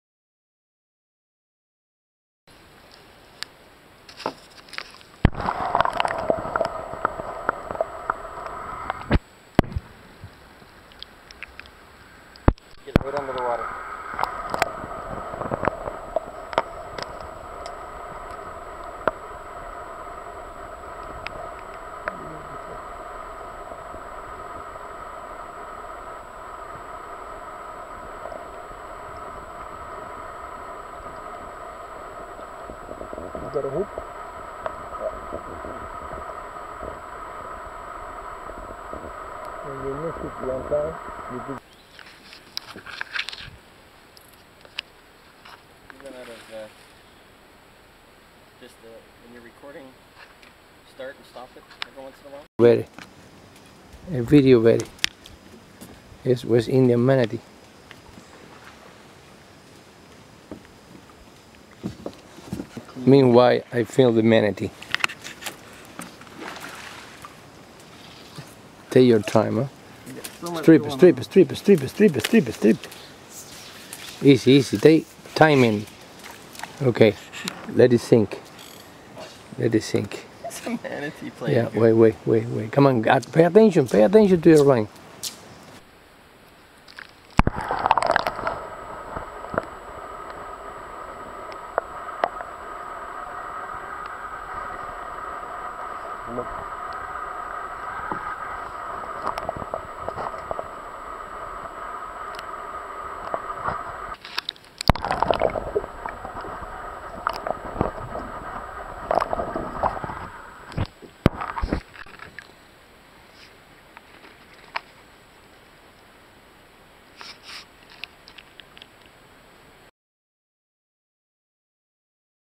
Manatee watching us on the Rio Hatiguanico, Cuba
While Tarpon fishing on the Rio Hatiguanico in Zapata National Park,
Cuba we were pleasantly surprised by this very curious and inquisitive
visitor.

She followed us all over this little tributary.